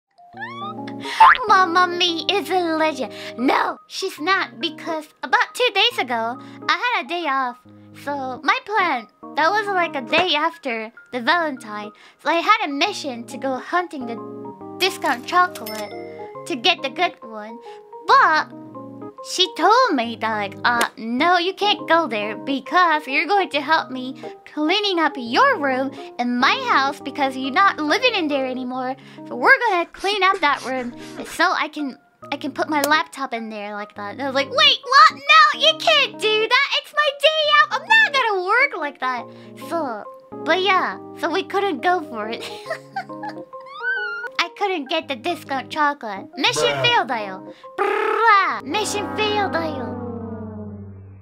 Mamamee ruined Pikamee's plans [Amano Pikamee]
Pikamee wanna go for the discount chocolate, but...